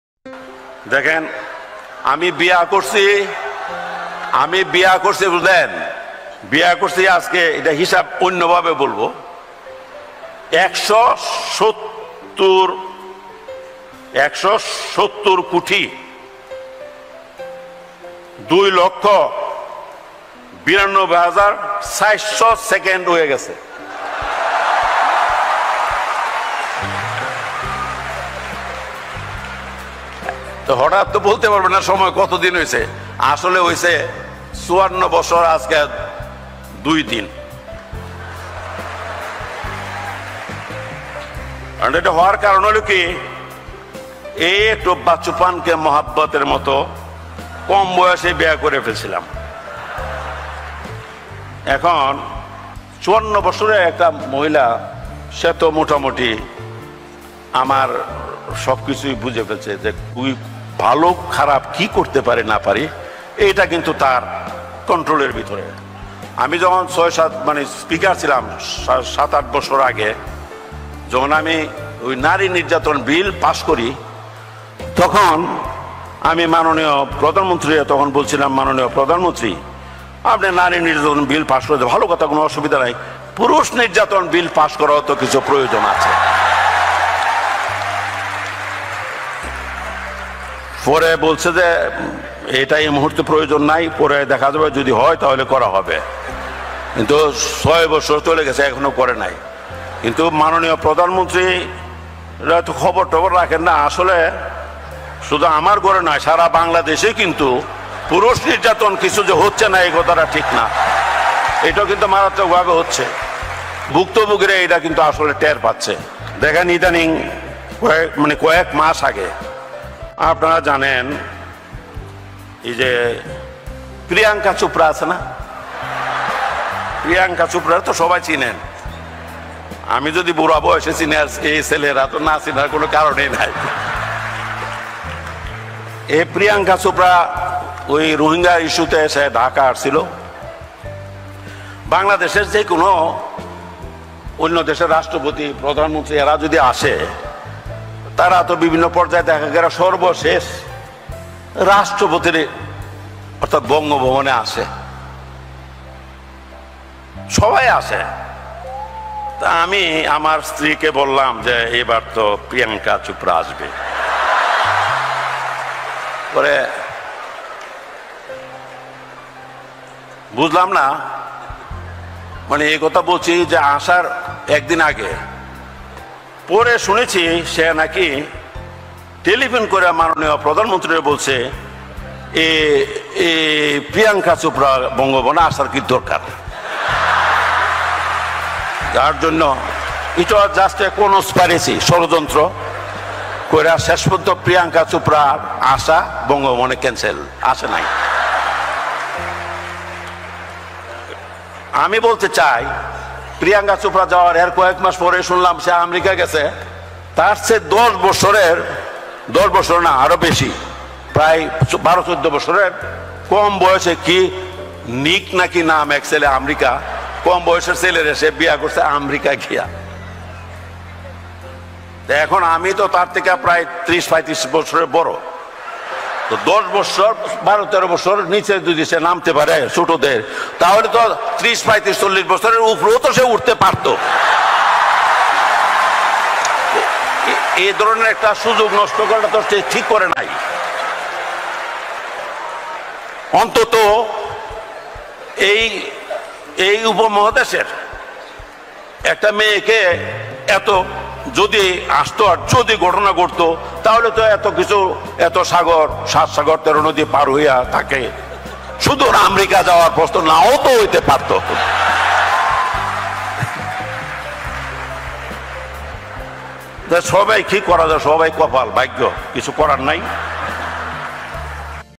রাষ্ট্রপতির হাসির বক্তব্য
বাংলাদেশের ৩ বারের রাষ্ট্রপতি আব্দুল হামিদ